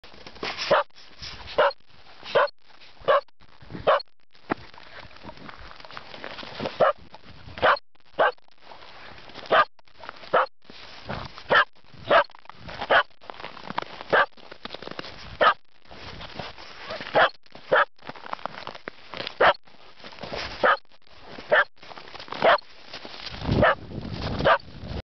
Sheltie ice fun!
Kibbles the sheltie playing on the ice at the park. He's very barky when he's not on leash, expecting me to run with him. unfortunatly I tend to fall over while running on ice :)

He is wearing a shirt because its freezing out, and it helps with the arm pit snow balls.